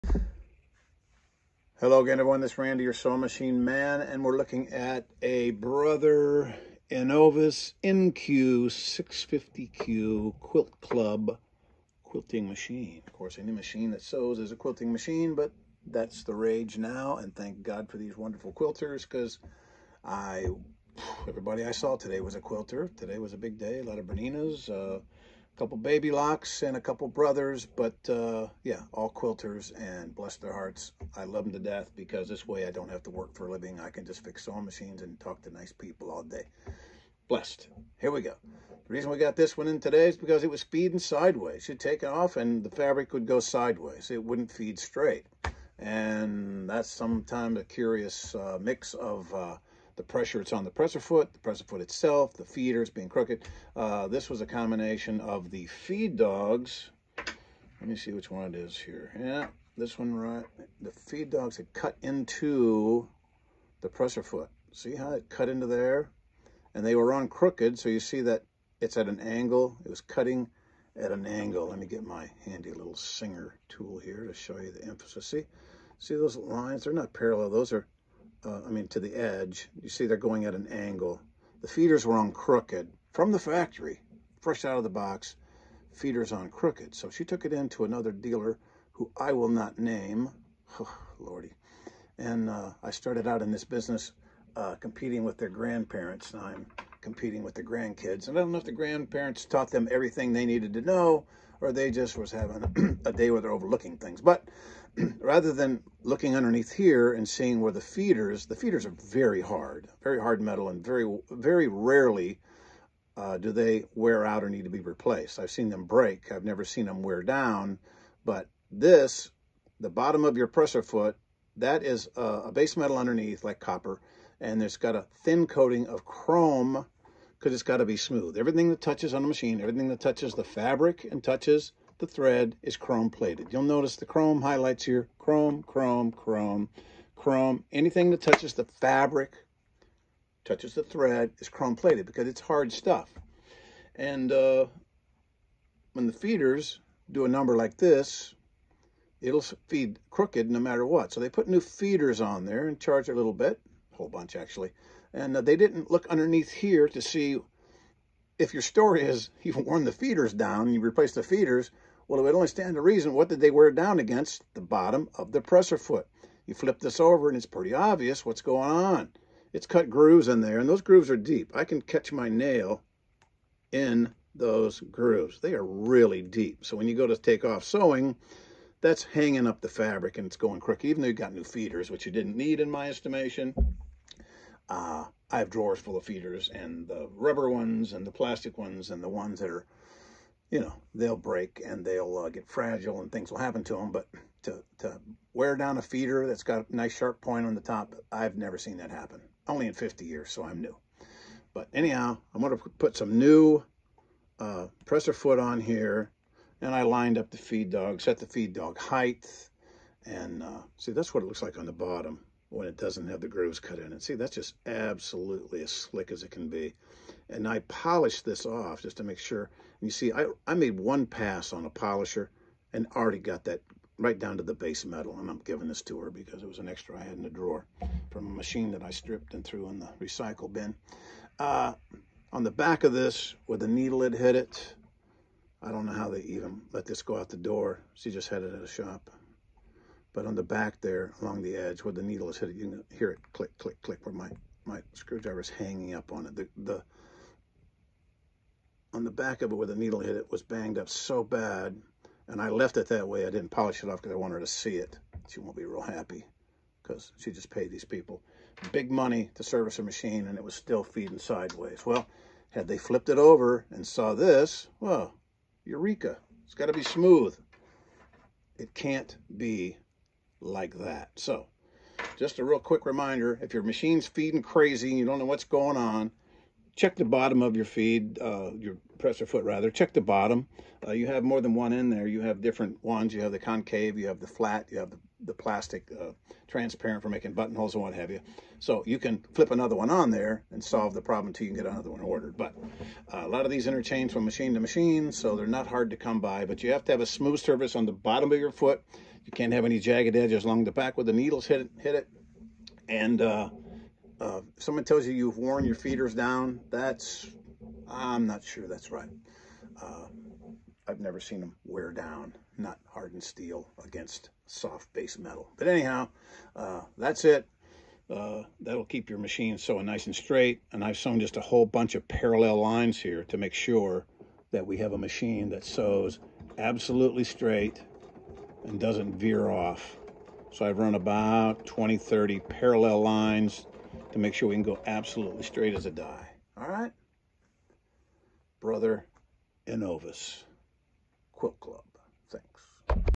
Feeding Crooked-Brother NX-650Q-grooves cut in the presser foot Easy Fix, get a new foot (video 334)
The presser foot on most new machines will need to be replaced frequently if you sew a lot. The chrome plating is super thin. I would say it’s like it was painted on, but it’s thinner than that.